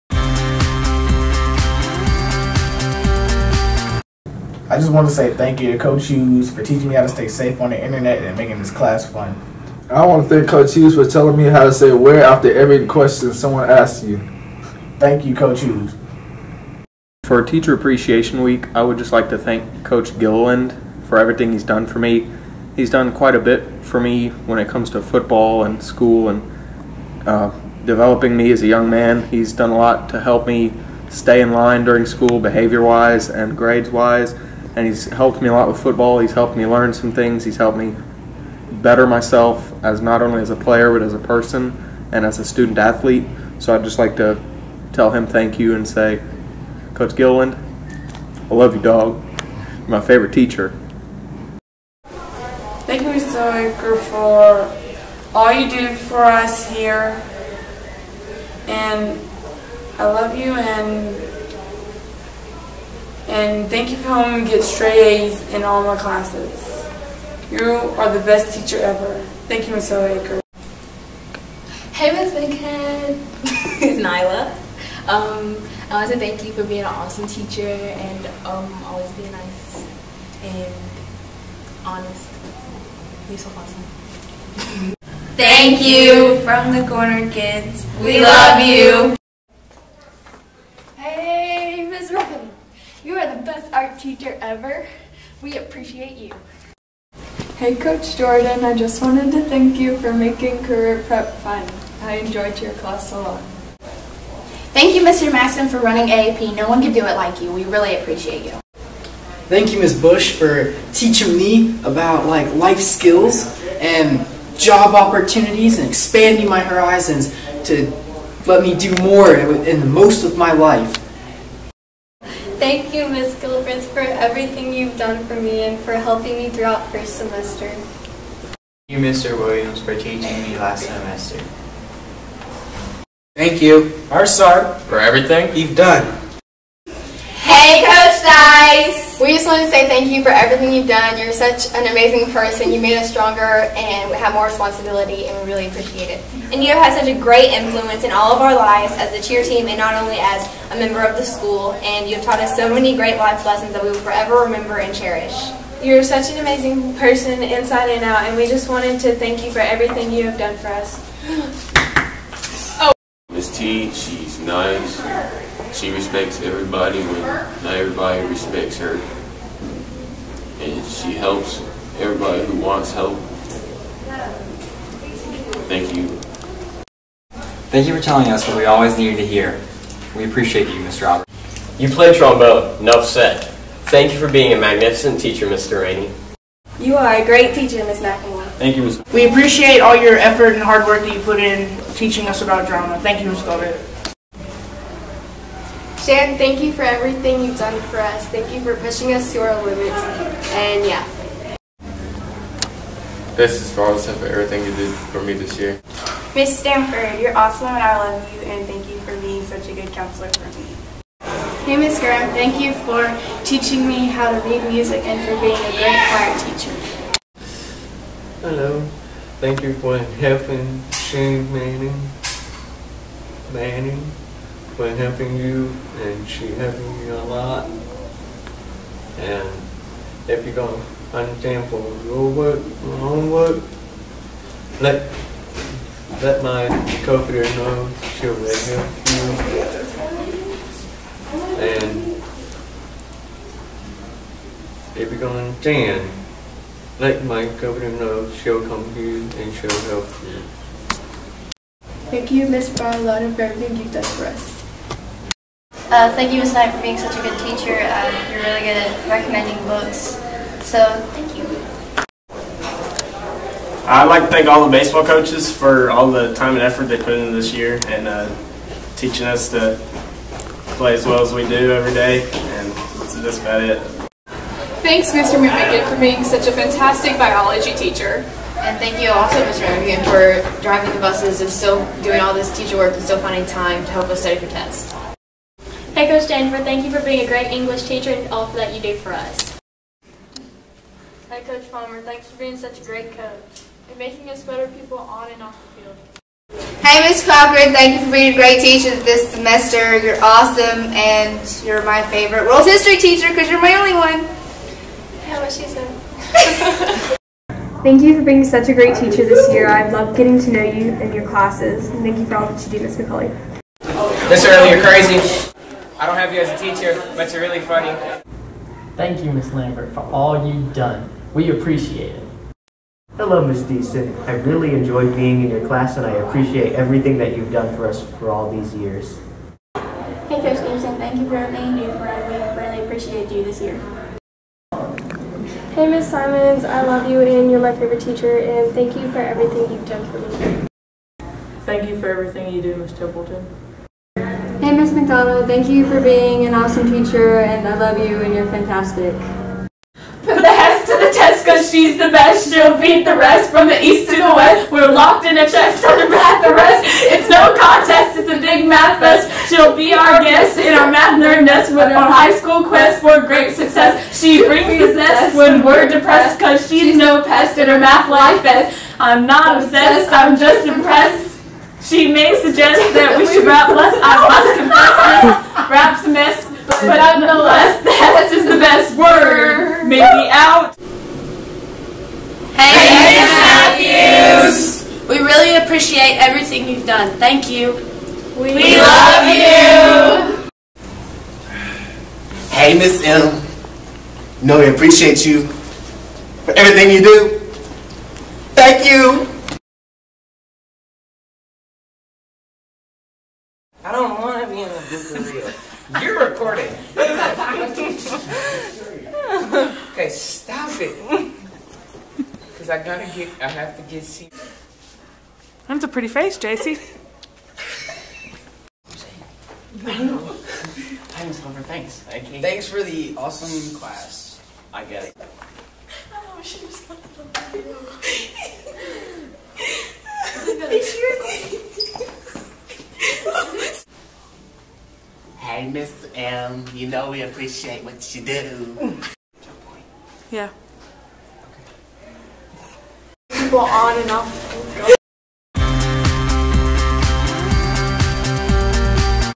S9 Teacher Appreciation 2014
S9 Students would like to thank their 9th Grade Teachers